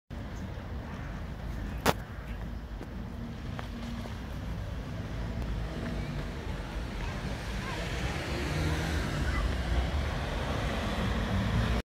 1867 - REG VA57 KHE ... Back Again As Usual ...
REG B16 CEN ... SIGN WHAT SIGN ... What matters most above this unlawfully fly-pitched ice cream van's operative selling on a public highway ... has nothing at all to do with the wording of the ice cream sales prohibition sign ... IT'S ALL ABOUT CENJIZ'S PROFITS ... !!!
CENJIZ'S ACTIONS ARE THE QUINTESSENCE OF WILLFUL CONTEMPT OF PUBLIC INTERESTS ... REPLACED BY SELF-INTEREST £££ GREED  !!!